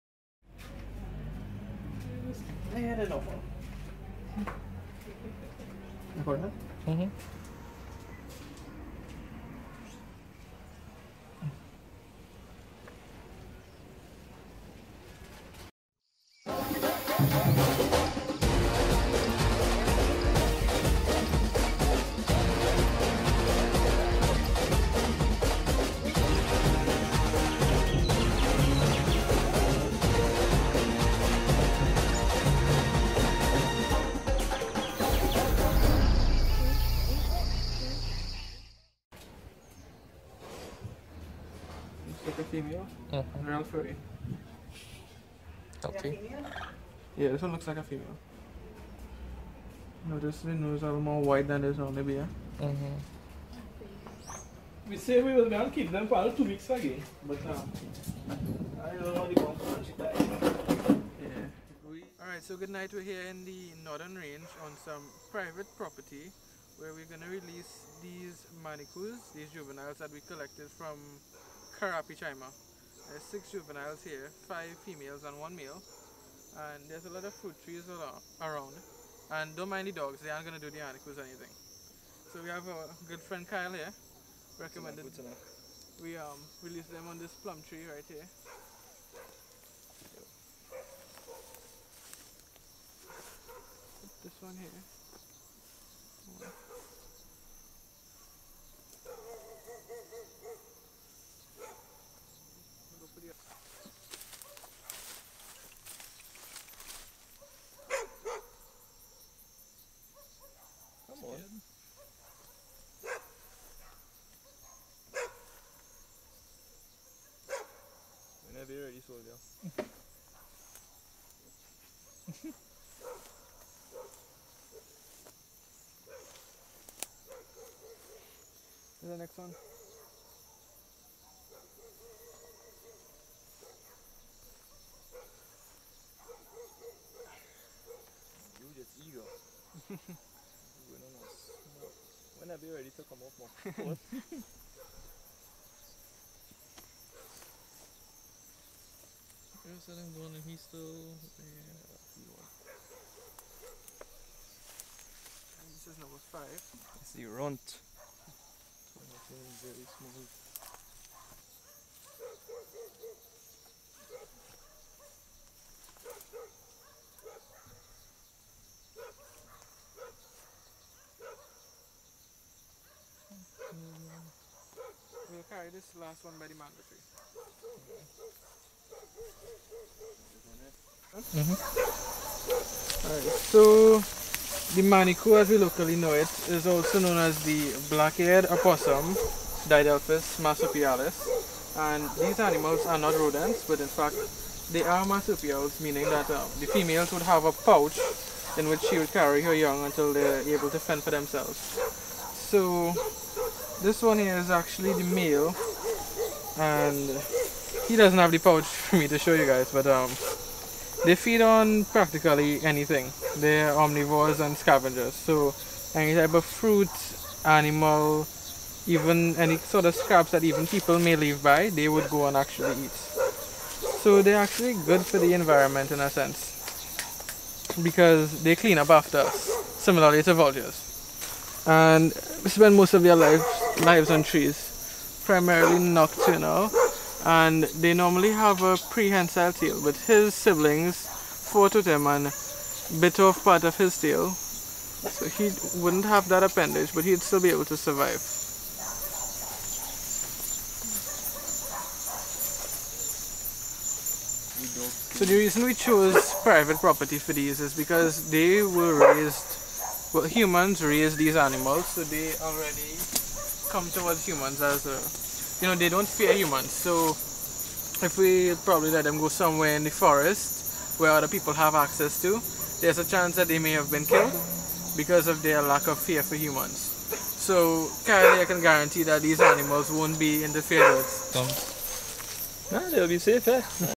Relocation of Manicous!
In this video we show the collection of 6 baby opossums from a family in Carapichima who had found a mommy opossum (who was named Manisha) with babies in their cat cage and they kept her together with the babies as pets, unfortunately the mother opposum passed away and the family weren't able to take care of all the babies.

We collected and looked after the children for a few days to make sure they were able to fend for themselves out in the wild.

We released these in a private estate where our friend Kyle would be the only person they would encounter